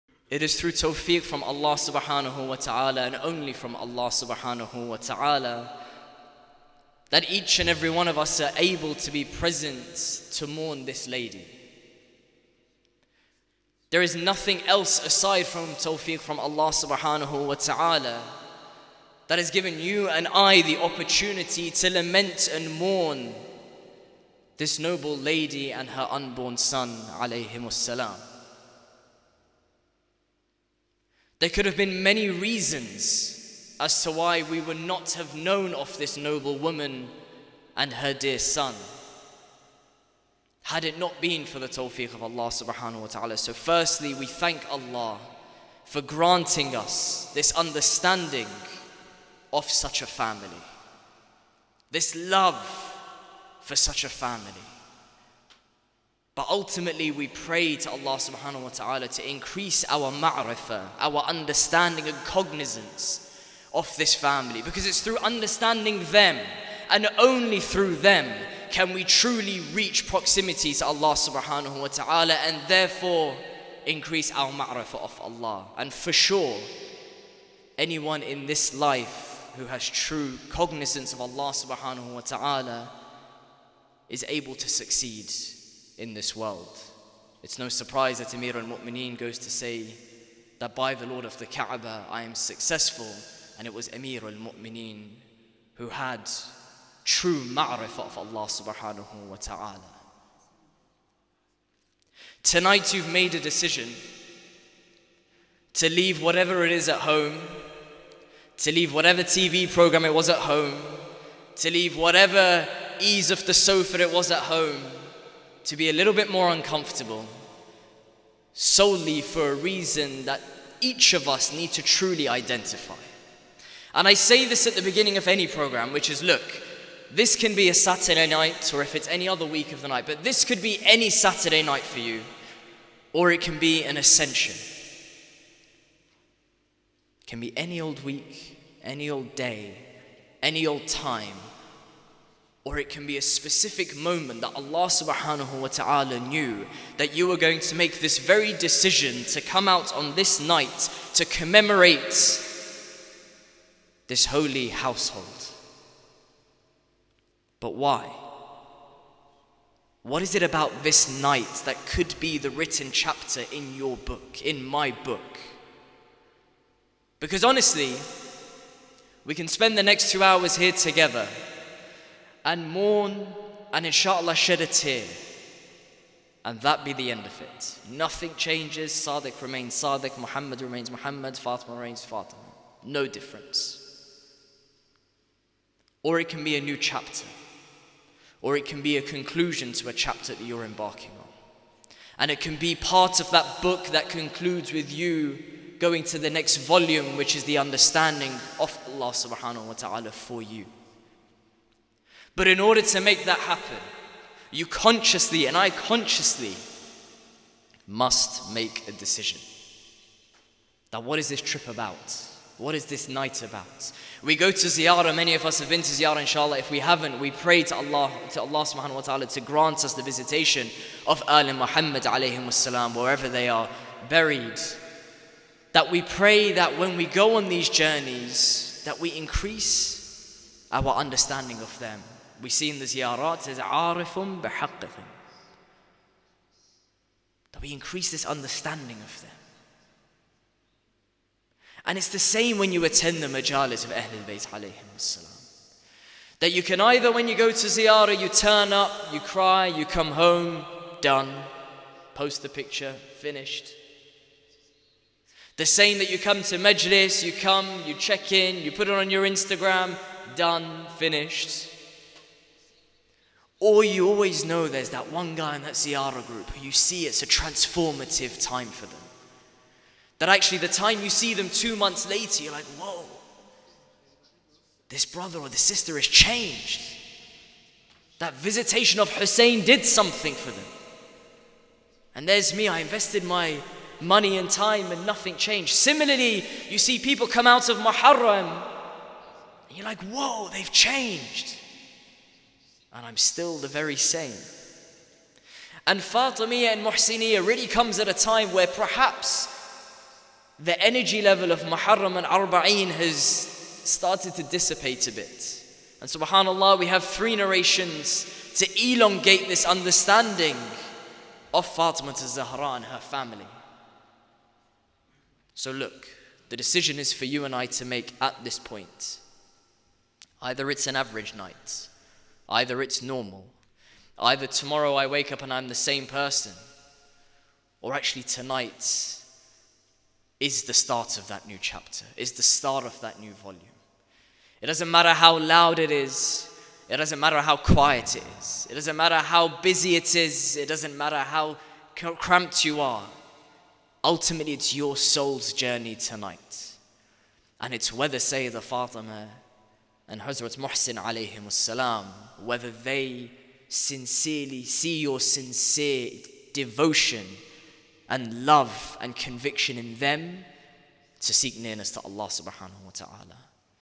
The blessing of being a Shi'a - Sadiq Damani at The Forgotten Child 1441/2020
May Allah (swt) make us all amongst those who have truly comprehended the message of the Ahlulbayt (as) and make us all firm as their followers.